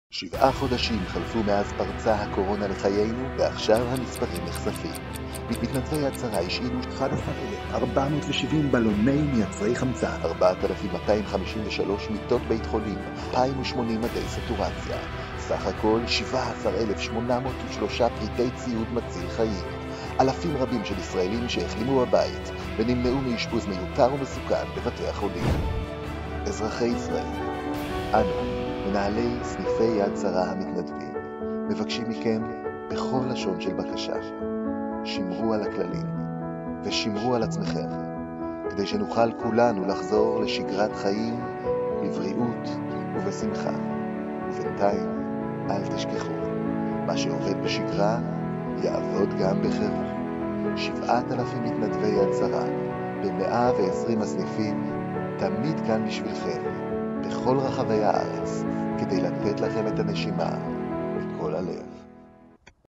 יד שרה - הנתונים המלאים נחשפים
7 חודשים חלפו מאז פרצה הקורונה לחיינו.
עכשיו נחשפים הנתונים המלאים, שמנעו את קריסת מערכת הבריאות בישראל.